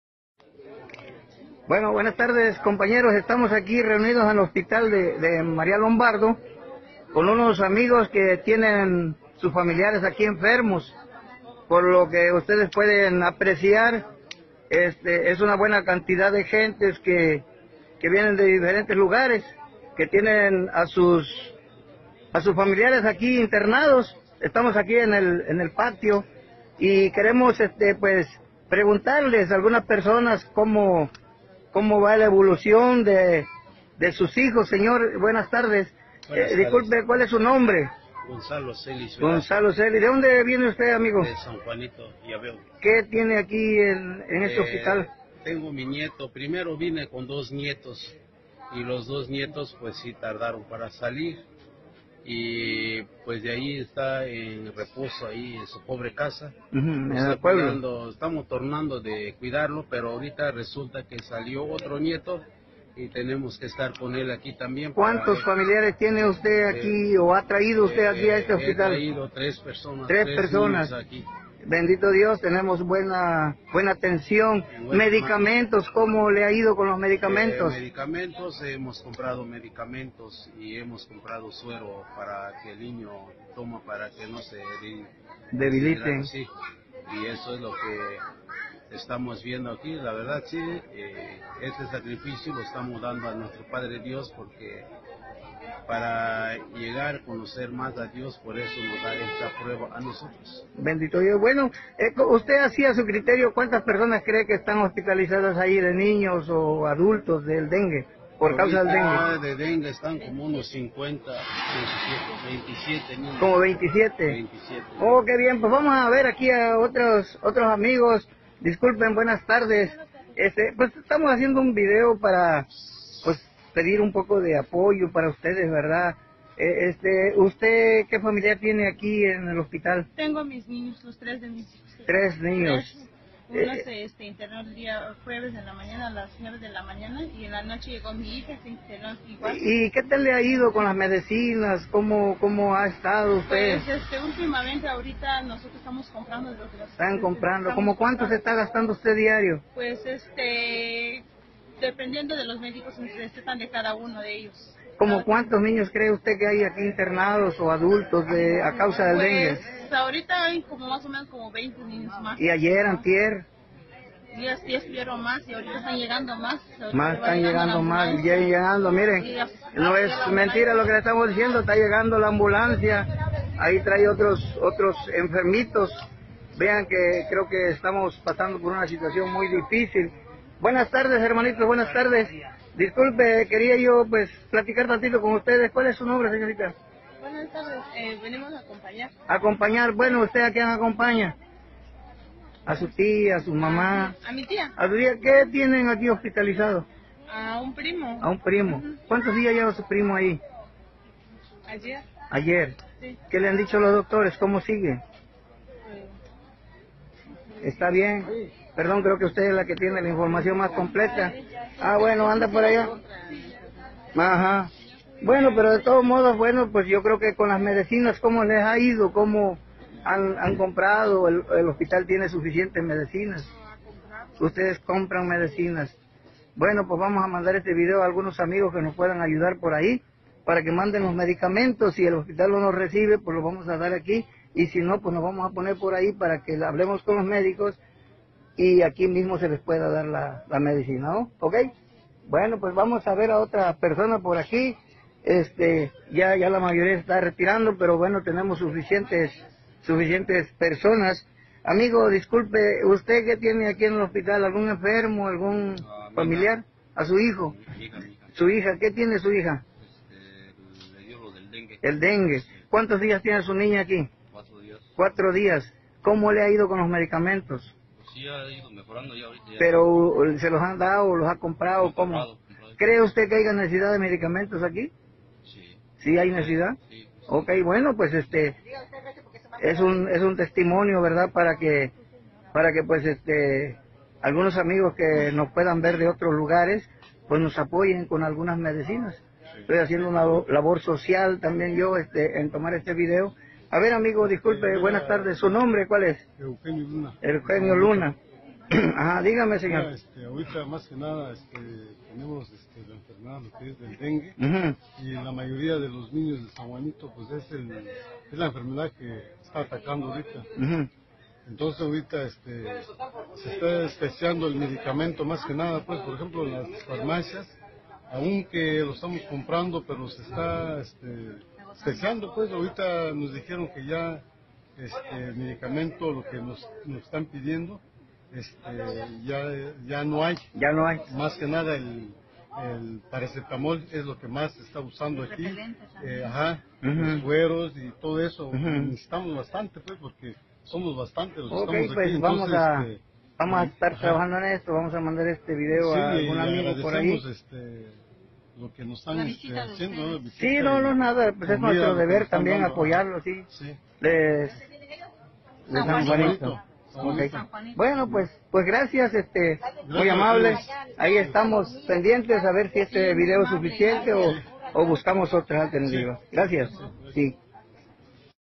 Habitantes de San Juan Jaltepec denuncian falta de medicamentos en hospital
Habitantes de San Juan Jaltepec, municipio de Santiago Yaveo; dan testimonio de lo que se vive en esa población ante el aumento de casos de Dengue Hemorrágico que está afectando principalmente a niños; ante esto, solicitan el apoyo de la ciudadanía para adquirir medicamentos, pues aseguran que no se cuenta con ellos en el nosocomio de zona.